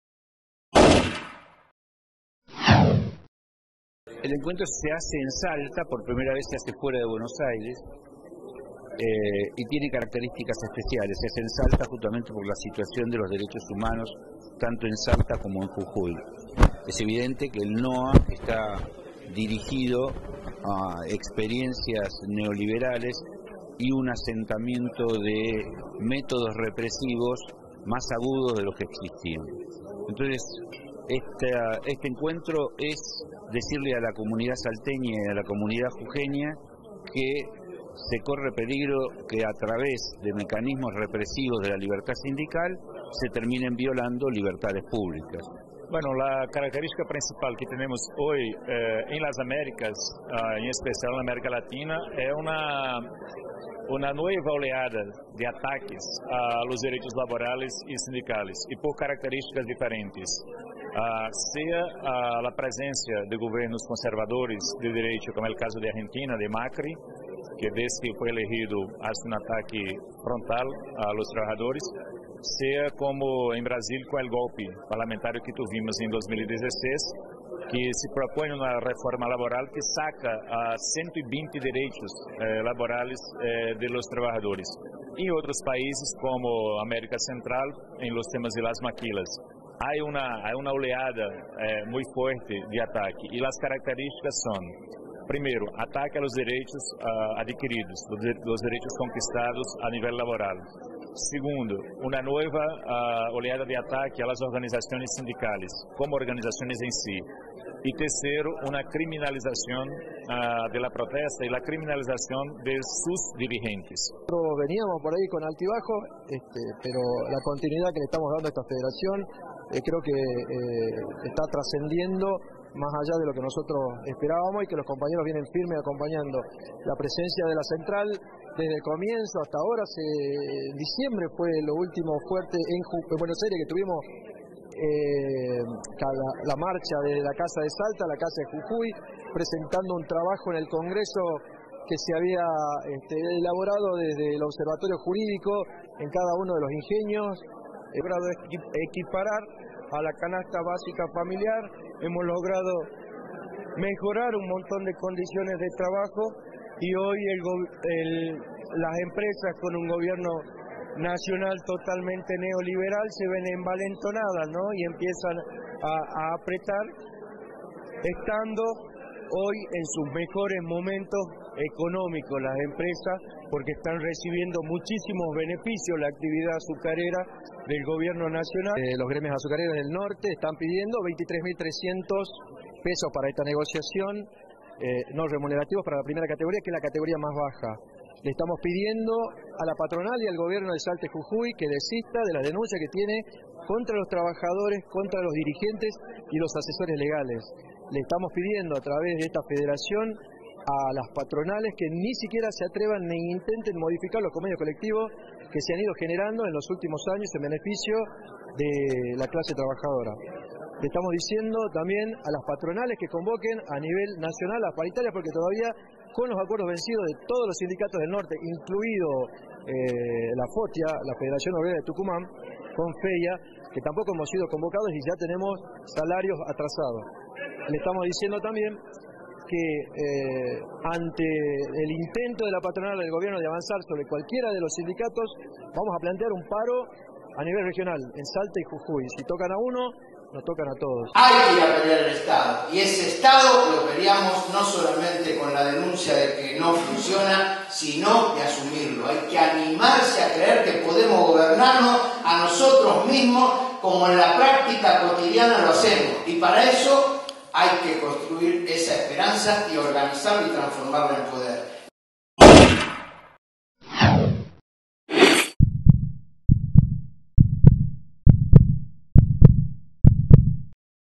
XVII Encuentro de Abogados de la CTA Autónoma
XVII Encuentro de Abogados de la CTA Autónoma tuvo como objetivo caracterizar esta nueva ola represiva que puede verse con particular crudeza en el NOA y fortalecer las intervenciones de los equipos jurídicos para enfrentar este proceso. Represión, detenciones arbitrarias, criminalización de dirigentes y trabajadores, persecución a sindicatos, tutelas sindicales amenazadas, multas son algunas de las variantes que se registran en el recrudecimiento de la criminalización de las protestas y el hostigamiento a los trabajadores y sus formas de organización en la zona.